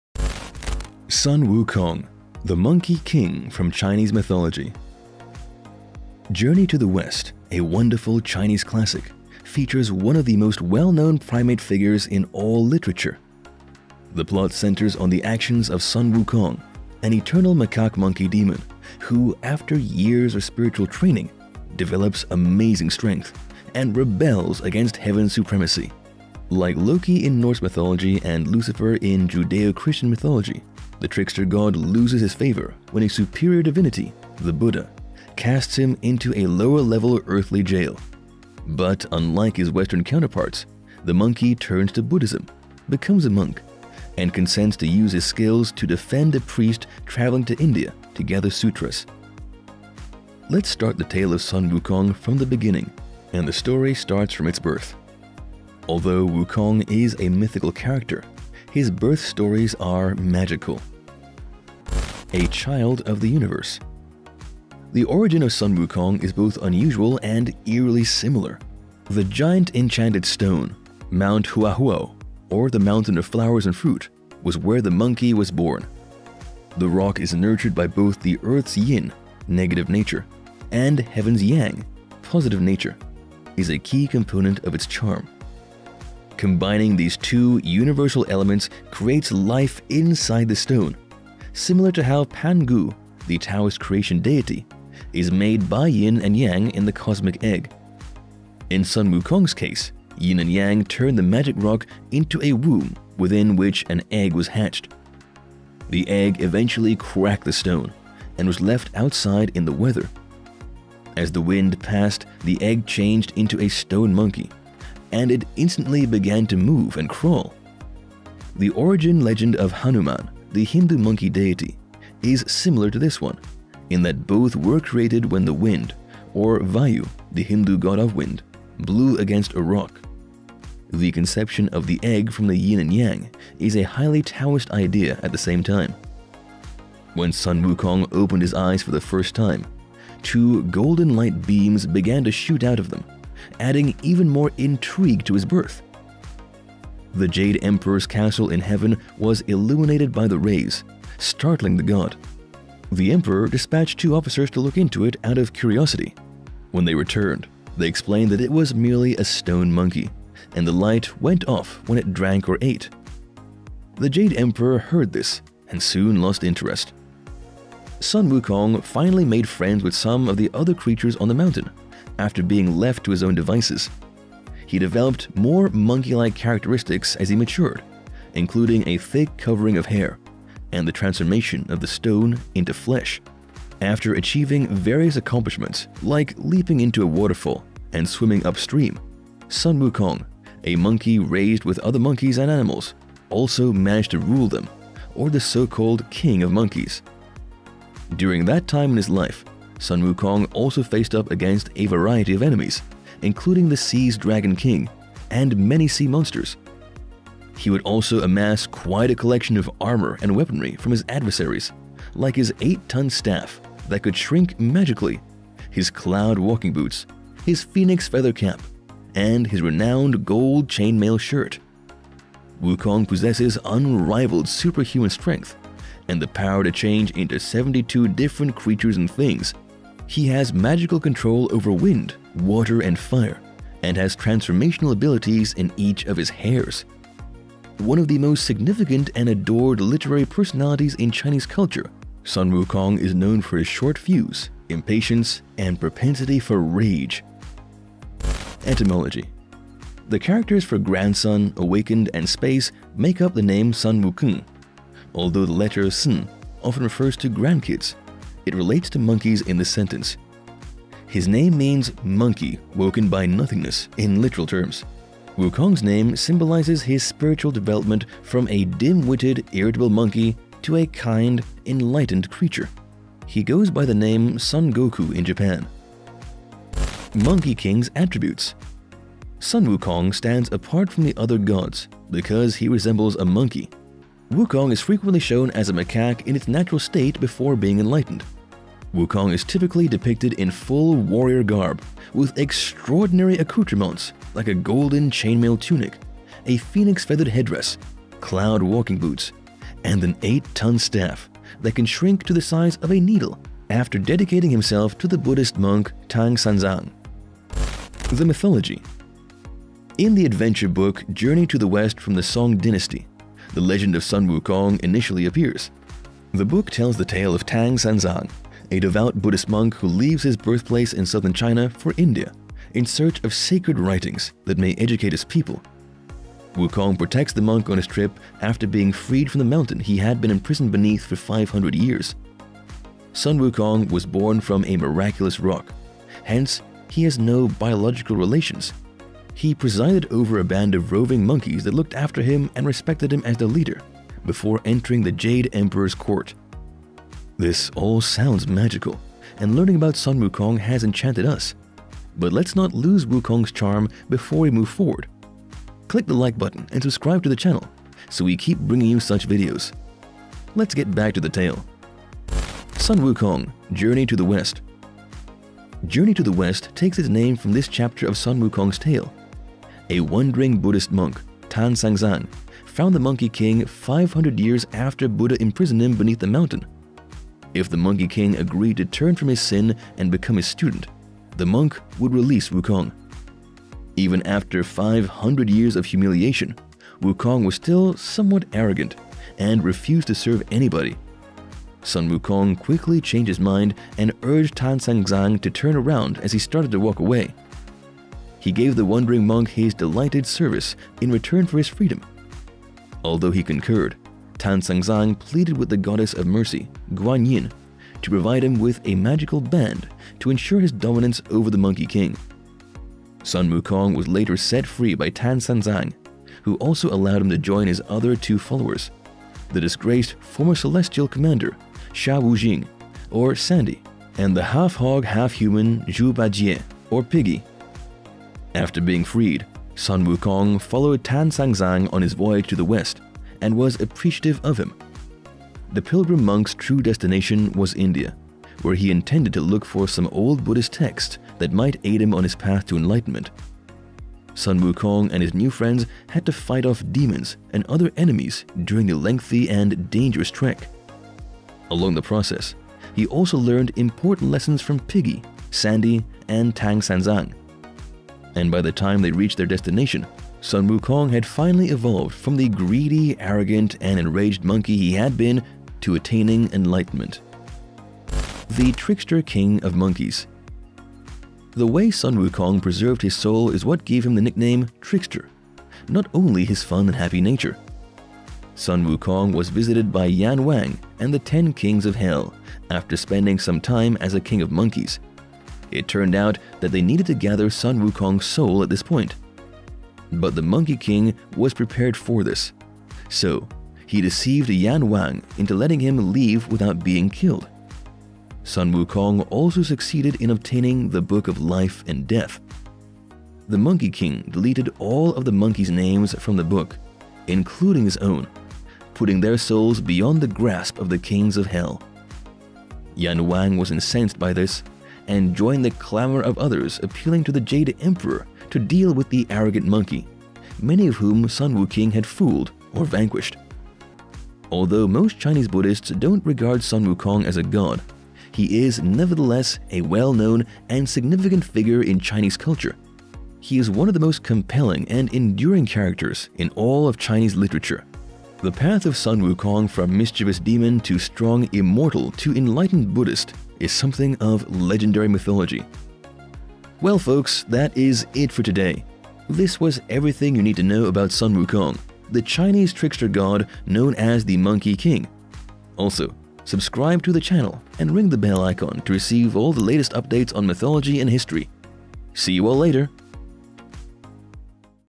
Sun Wukong: The IMMORTAL Monkey King. You WON'T believe who imprisoned him!
The monkey king acquired supernatural abilities and power by learning Taoist. One of the craziest but most magical characters in Mythology.

Journey to the West, a wonderful Chinese classic, features one of the most well-known primate figures in all literature. The plot centers on the actions of Sun Wukong, an eternal macaque monkey demon who, after years of spiritual training, develops amazing strength and rebels against Heaven's supremacy. Like Loki in Norse mythology and Lucifer in Judeo-Christian mythology, this trickster god loses his favor when a superior divinity, the Buddha, casts him into a lower-level earthly jail. But unlike his Western counterparts, the Monkey turns to Buddhism, becomes a monk, and consents to use his skills to defend a priest traveling to India to gather sutras.

Let's start the tale of Sun Wukong from the beginning, and ay story starts from its birth. Although Wukong is a mythical character, his birth stories are magical.

The origin of Sun Wukong is both unusual and eerily similar. The giant enchanted stone, Mount Huahuo, or the Mountain of Flowers and Fruit, was where the Monkey was born. The rock is nurtured by both the Earth's yin (negative nature) and Heaven's yang (positive nature) is a key component of its charm.

Combining these two Universal elements creates life inside the stone, similar to how Pan Gu, the Taoist creation deity, is made by yin and yang in the cosmic egg. In Sun Wukong's case, yin and yang turned the magic rock into a womb within which an egg was hatched.

The egg eventually cracked the stone and was left outside in the weather. As the wind passed, the egg changed into a stone monkey, and it instantly began to move and crawl. The origin legend of Hanuman, the Hindu monkey deity, is similar to this one in that both were created when the wind (or Vayu, the Hindu God of Wind) blew against a rock. The conception of the egg from the yin and yang is a highly Taoist idea at the same time.

When Sun Wukong opened his eyes for the first time, two golden light beans began to shoot out of them, adding even more intrigue to his birth. The Jade Emperor's castle in Heaven was illuminated by the rays, startling the god. The emperor dispatched two officers to look into it out of curiosity. When they returned, they explained that it was merely a stone monkey, and the light went off when it drank or ate. The Jade Emperor heard this and soon lost interest.

Sun Wukong finally made friends with some of the other creatures on the mountain after being left to his own devices. He developed more monkey-like characteristics as he matured, including a thick covering of hair and the transformation of the stone into flesh. After achieving various accomplishments, like leaping into a waterfall and swimming upstream, Sun Wukong, a monkey raised with other monkeys and animals, also managed to rule them or the so-called King of Monkeys.

During that time in his life, Sun Wukong also faced up against a variety of enemies, including the sea's Dragon King and many sea monsters. He would also amass quite a collection of armor and weaponry from his adversaries, like his eight-ton staff that could shrink magically, his cloud-walking boots, his phoenix feather cap, and his renowned gold chainmail shirt.

Wukong possesses unrivaled superhuman strength and the power to change into 72 different creatures and things. He has magical control over wind, water, and fire and has transformational abilities in each of his hairs. One of the most significant and adored literary personalities in Chinese culture, Sun Wukong is known for his short fuse, impatience, and propensity for rage.

🔥KEEP WATCHING FOR THE REST OF THE STORY...